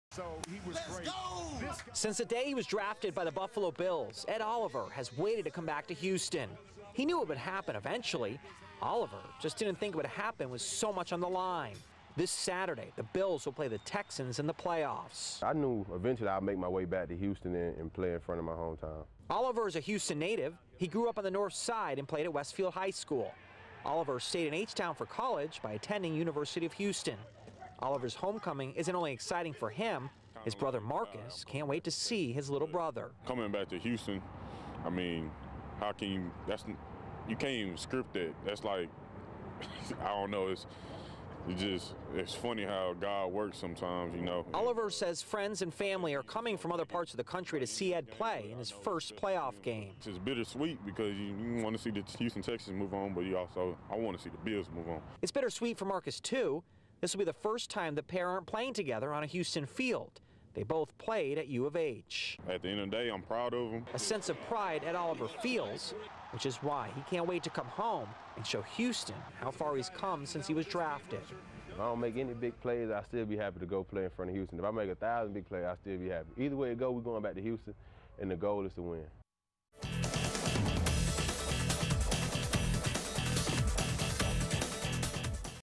How Ed Oliver feels about playing against his hometown in the playoffs
Since the day he was drafted by the Buffalo Bills, Ed Oliver has waited to come back to Houston. He knew it would happen eventually.